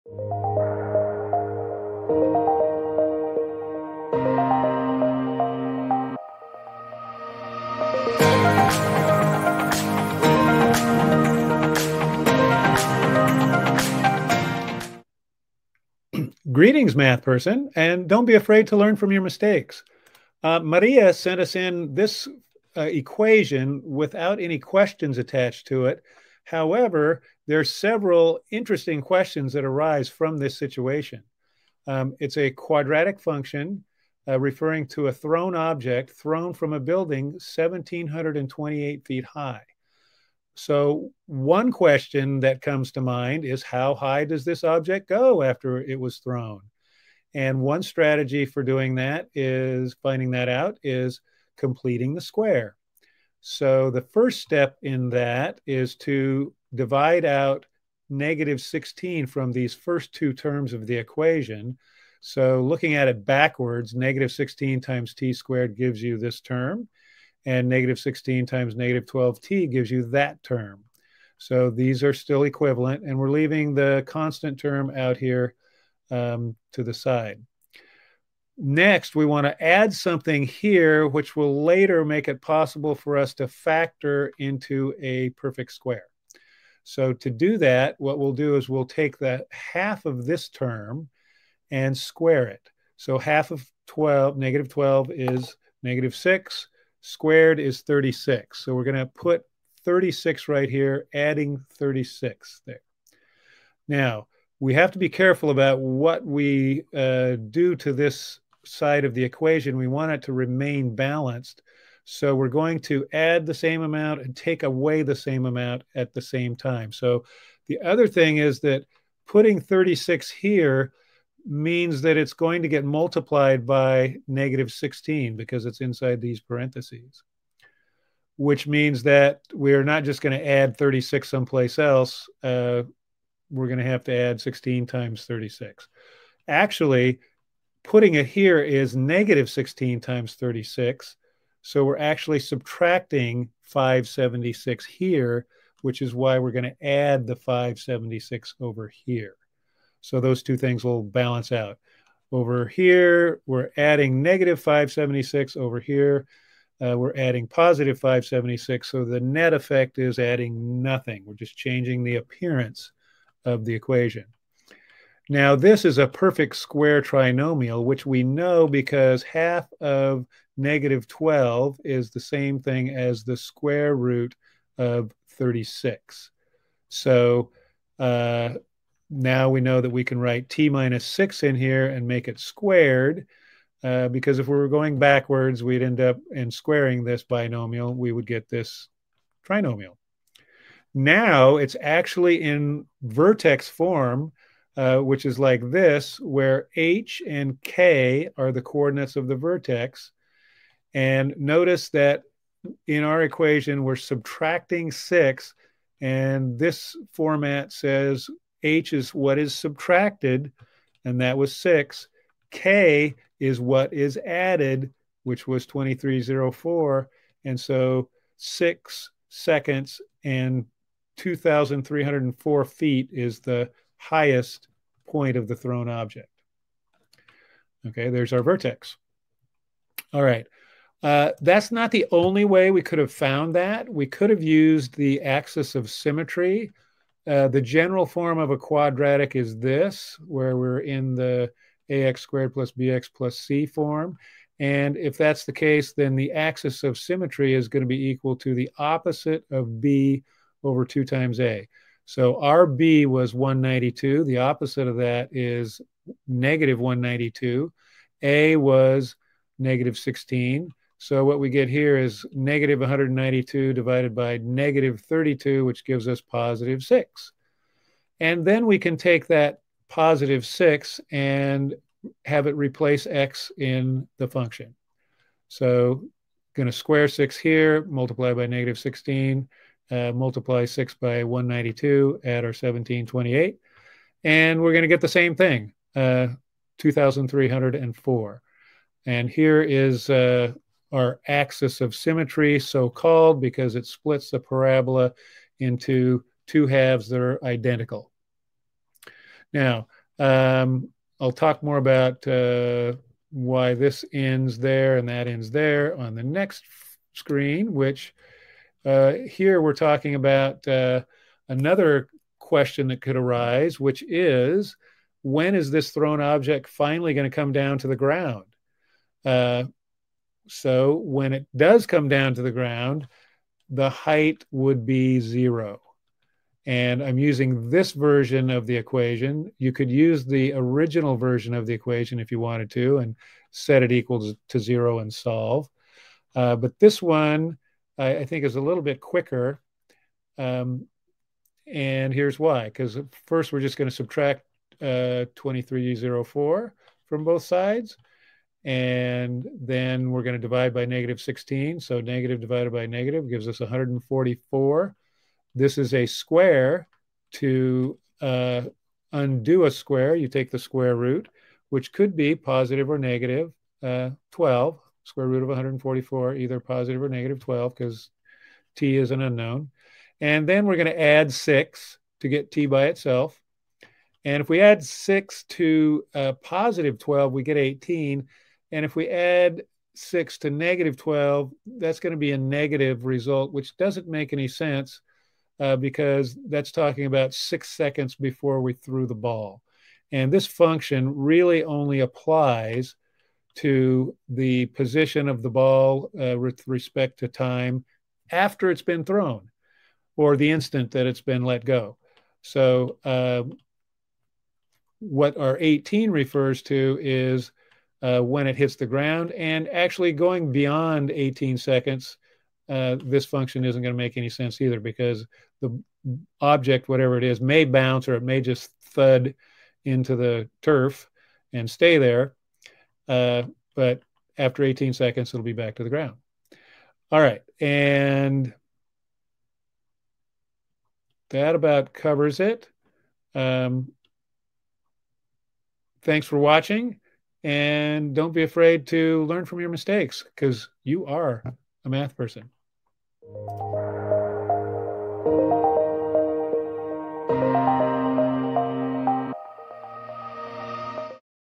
Maria's Projectile (Thrown Object)
Question: algebra1 , Algebra2
We throw an object upward from the top of a 1728 feet tall building. The height h of the object, (measured in feet) t seconds after we threw it ish=−16t2+192t+1728h=-16t2+192t+1728

Answered By:

Cary M.
Patient and Experienced Geometry Tutor